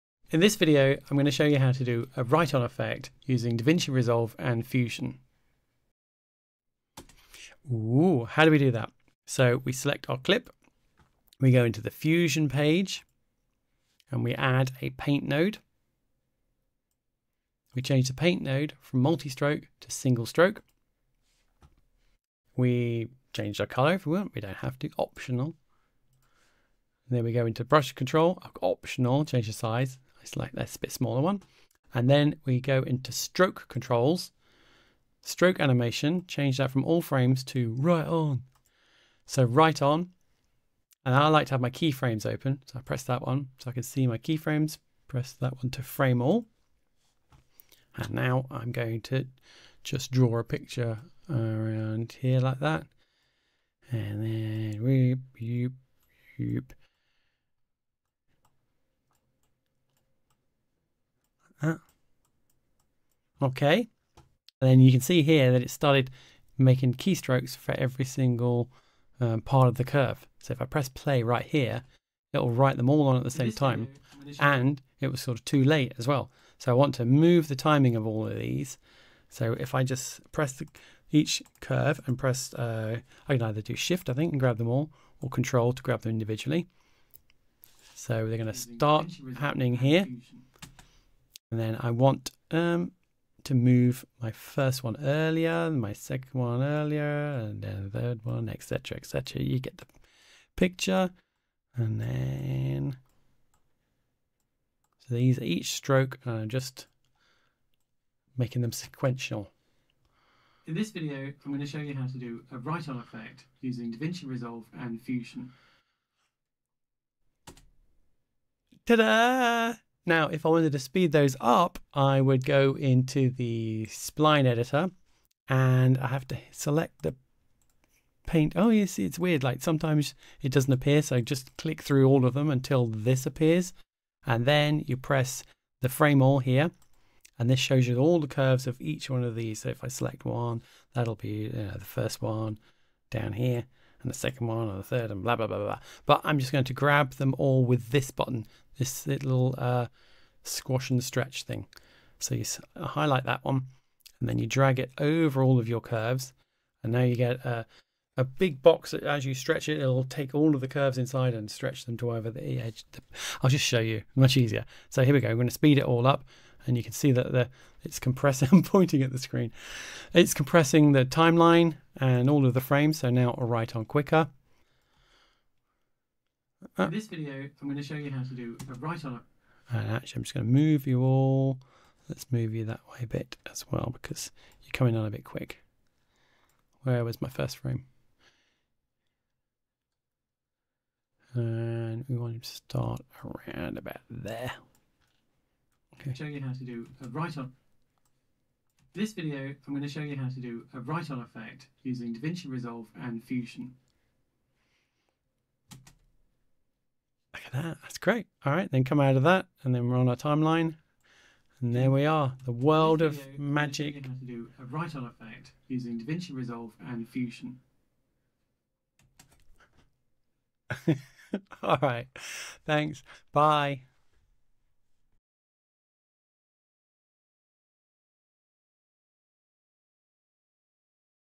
DRAW ON TOP of your videos using DaVinci Resolve's PAINT NODE (Tutorial)
This tutorial will show you how you can draw, write, or doodle on top of your videos using just Davinci Resolve Fusion. No iPad, no green screen, just a mouse 🐭 eeek!

The Paint node in Davinci Fusion allows the paint strokes to form over time, so they will animate on, and I'll show you how to adjust their timing. Fun Stuff! Nerd Alert : )

By day, I'm a full time Nuke compositor (and generalist) but I'm also learning to doing nuke type stuff in Davinci Fusion for home, so If you have suggestions for future videos please let me know in the comments. Thanks!